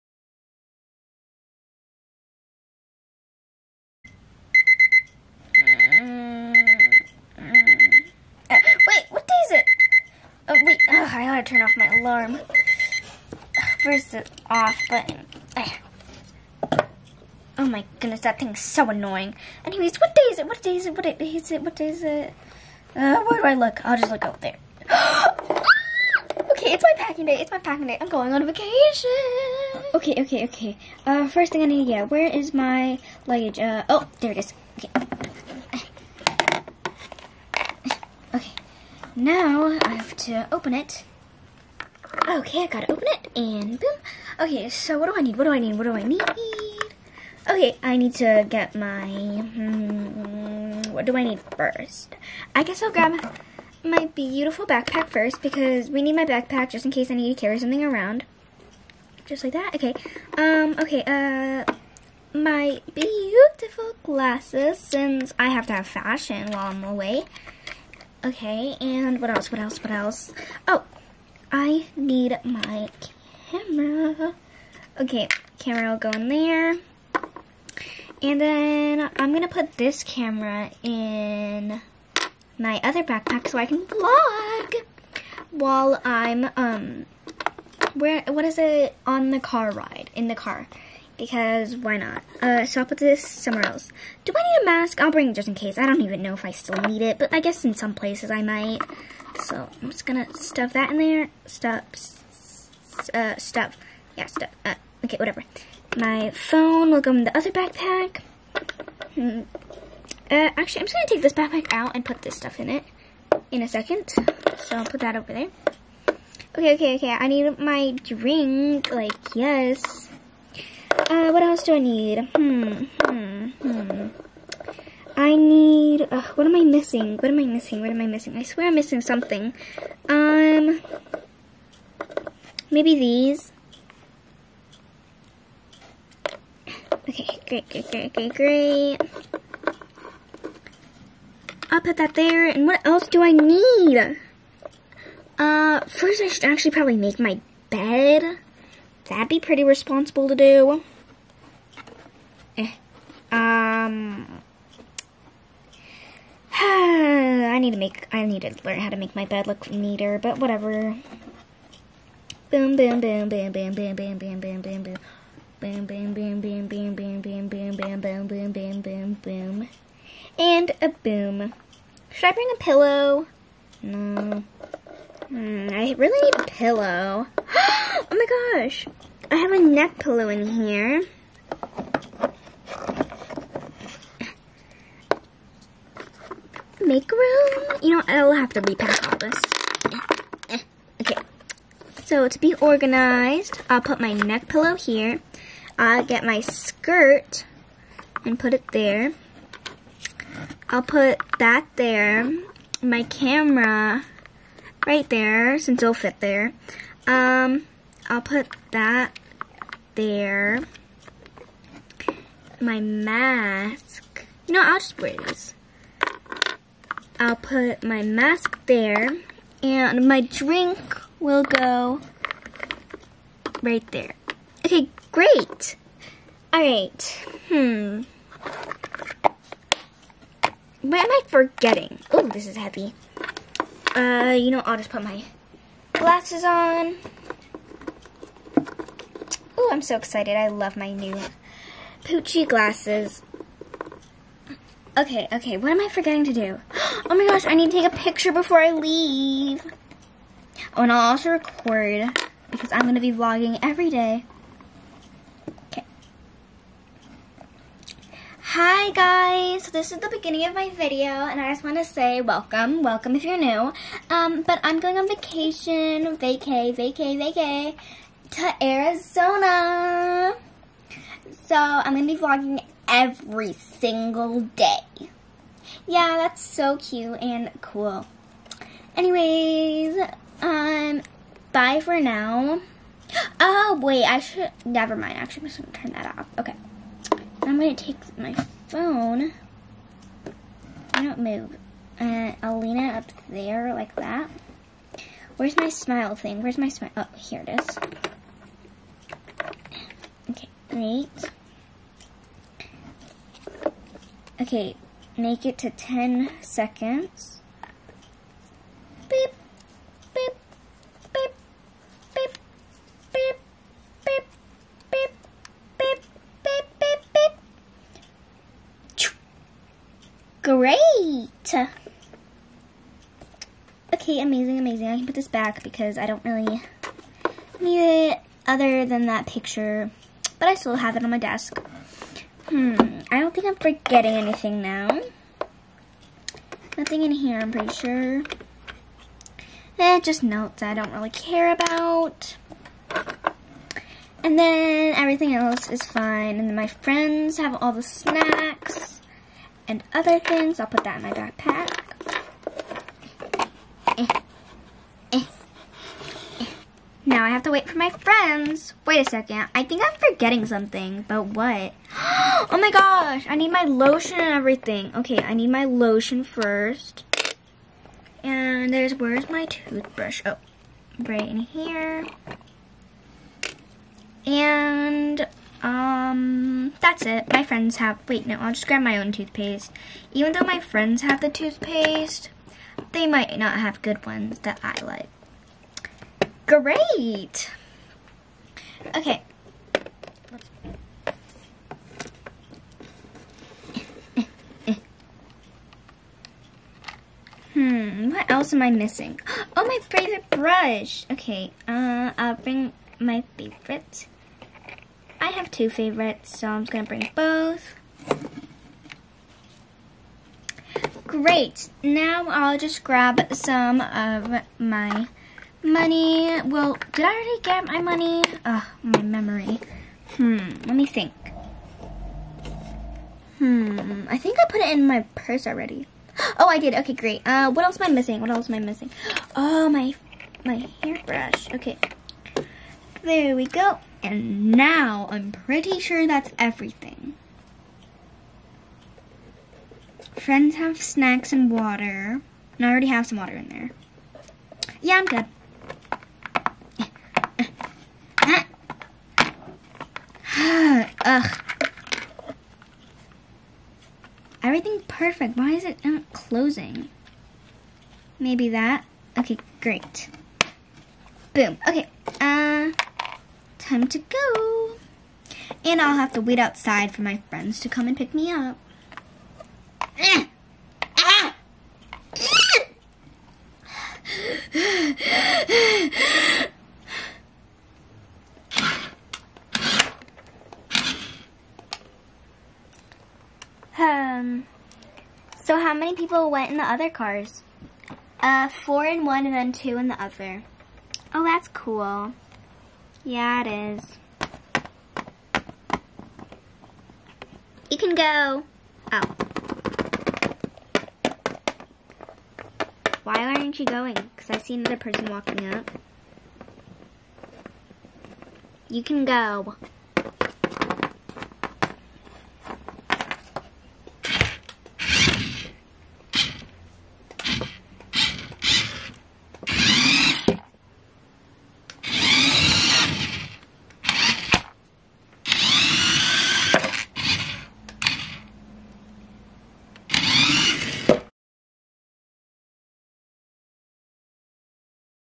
LPS Getting Ready For Vacation.
LPS packing for vacation to Arizona. When she finishes her friend comes and picks her up, they then start driving to Arizona.